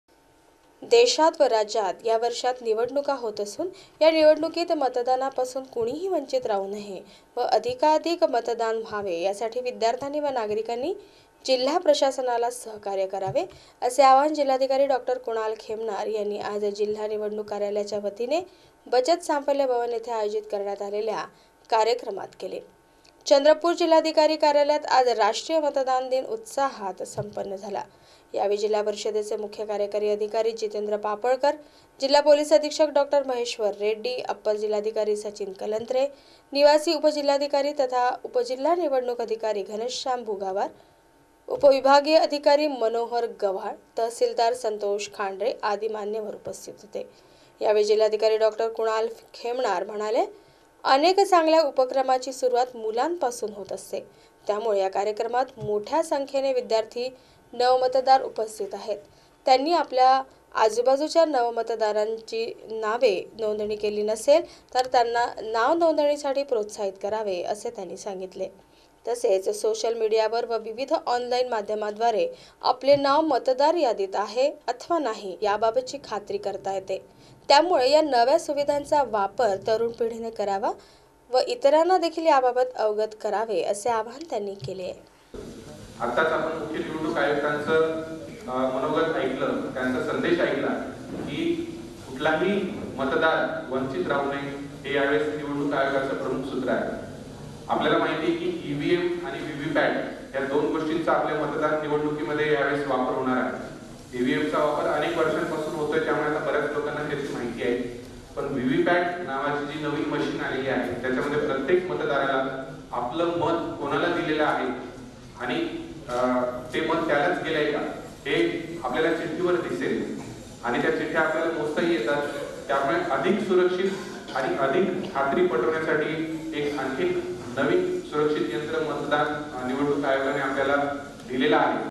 25 January 2019 Search Tv Chandrapur - जिल्हा निवडणूक कार्यालयाच्या वतीने रा
जिल्हा निवडणूक कार्यालयाच्या वतीने राष्ट्रीय मतदार दिन उत्साहात संपन्न